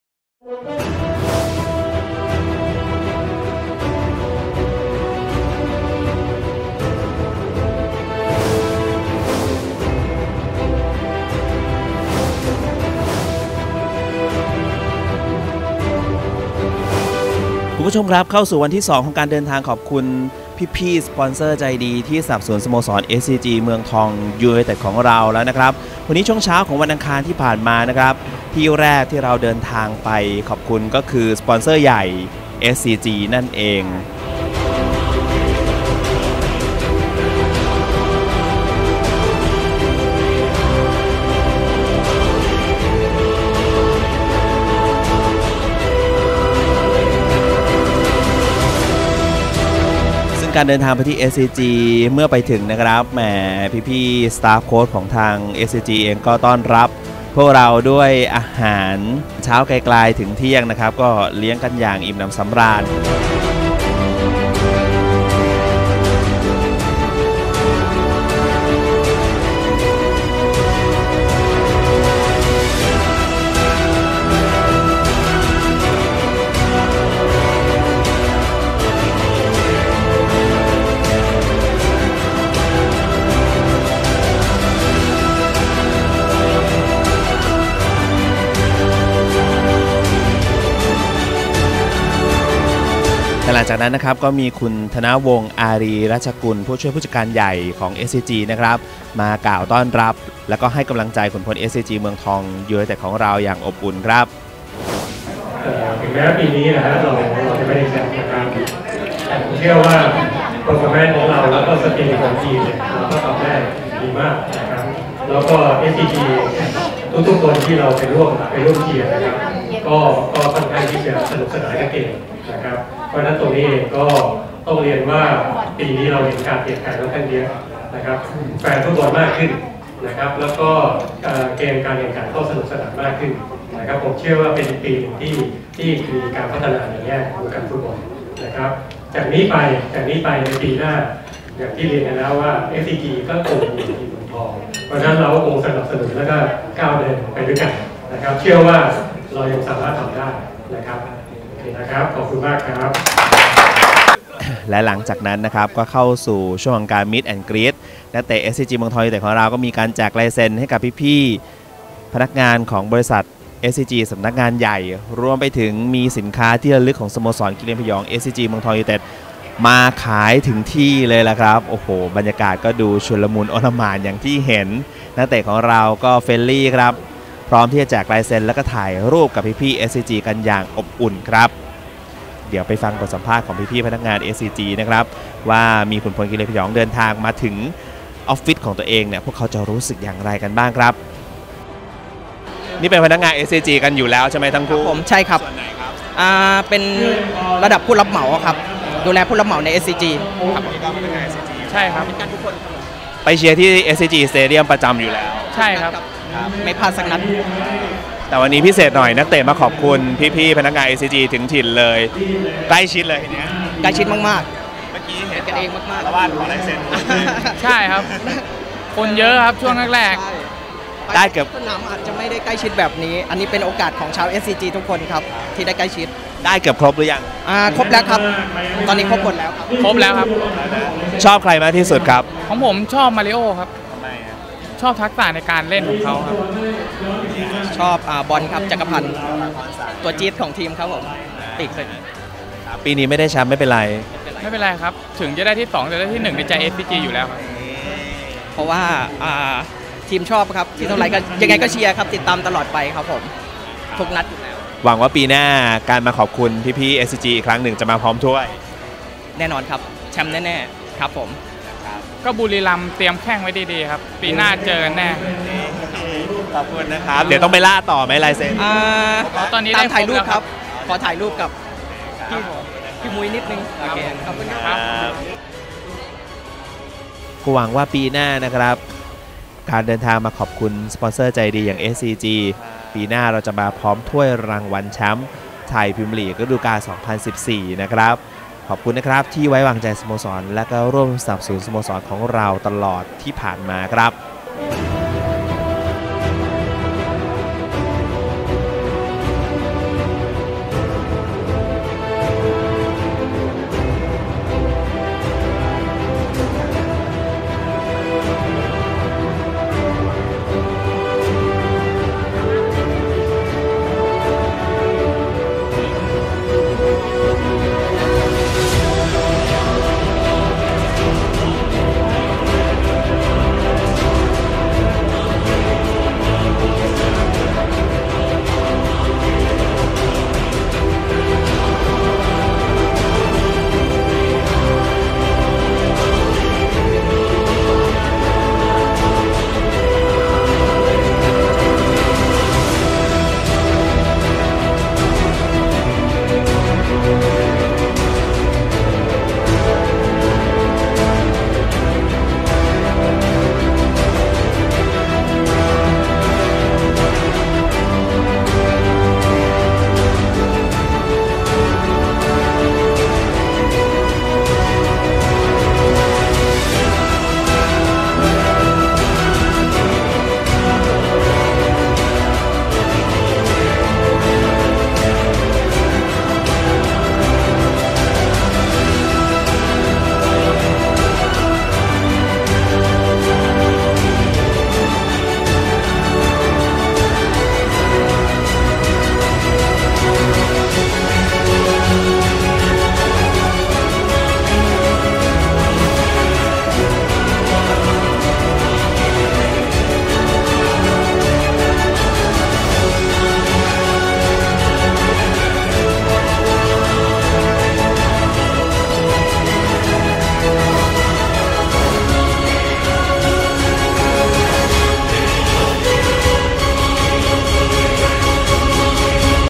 MTUTD.TV เดินทางไปขอบคุณ - SCG
ผู้บริหาร พร้อมด้วยนักเตะ "กิเลนผยอง" เอสซีจี เมืองทอง ยูไนเต็ด เดินทางขอบคุณผู้สนับสนุนหลักของทีมอย่าง บริษัท ปูนซีเมนต์ไทย จำกัด หรือ เอสซีจี ที่ให้การสนับสนุนสโมสรฯ เป็นอย่างดีตลอดฤดูกาลที่ผ่านมา ท่ามกลางการต้อนรับอย่างอบอุ่น จากผู้บริหารและพนักงาน